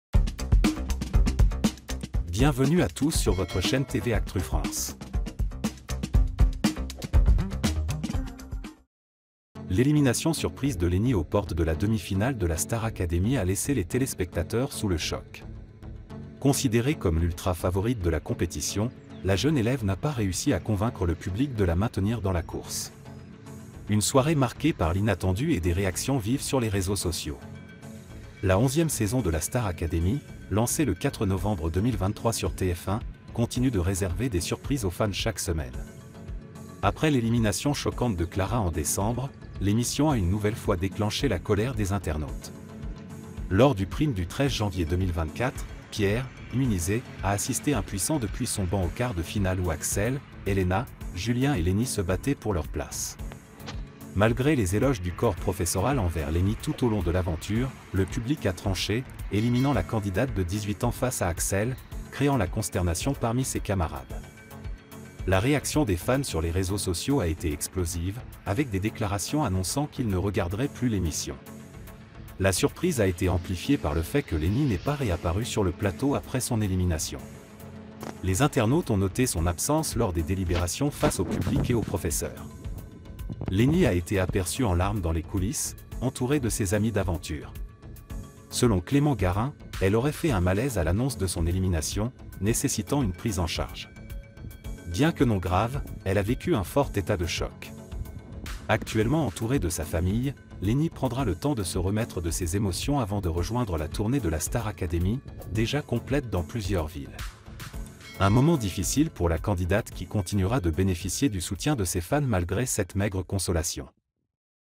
Lénie sous le choc après son élimination,des informations suggèrent un possible malaise en coulisses
L'élimination surprise de Lénie aux portes de la demi-finale de la Star Academy a laissé les téléspectateurs sous le choc. Considérée comme l'ultra favorite de la compétition, la jeune élève n'a pas réussi à convaincre le public de la maintenir dans la course.